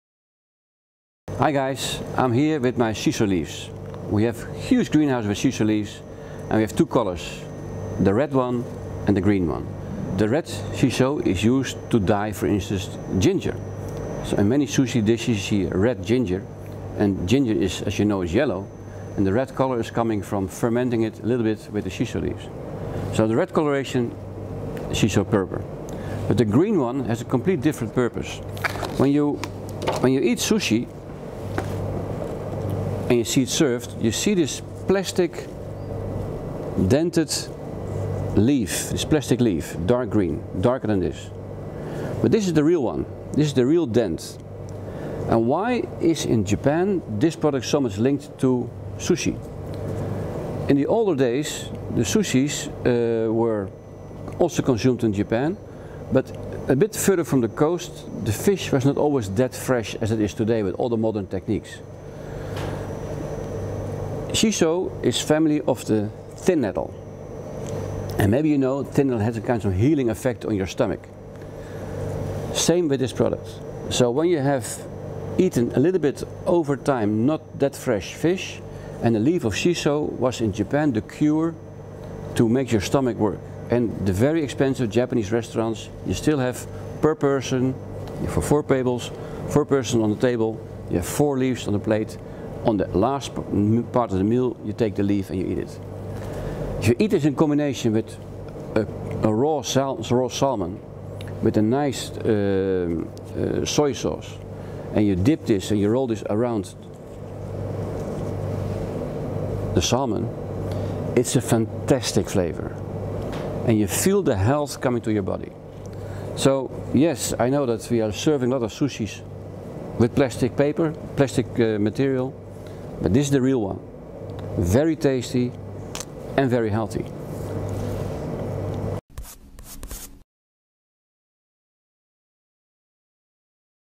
Rob Baan: "Shiso Leaves, fantastic with Sushi and Sashimi"
The very mild, but outspoken, taste with its strong fragrance makes it suitable to mix it into a salad. Take a small slice of fresh tuna, dip it in some Kikkoman soy sauce, wrap it into a Shiso Green Leaf and be amazed. A treat to your mouth. Try fresh tuna tartar mixed with fine shallots, some pepper and salt, a touch of soy sauce and some fresh chopped Shiso Green Leaves on top. In Asia it is popular as a Tempura. But as a variation we have fried a (well-dried with tissue paper) leaf in oil, and used it as decoration on a piece of meat. (dry it after frying on some paper and add some sea-salt. Shiso Leaves Green in Japan called ‘Oba’. There is no (raw) fish dish without one or more leafs. The main reason, next to its good taste, is the belief that the leaf is an intestine stimulant and protects against (mild) food poisoning.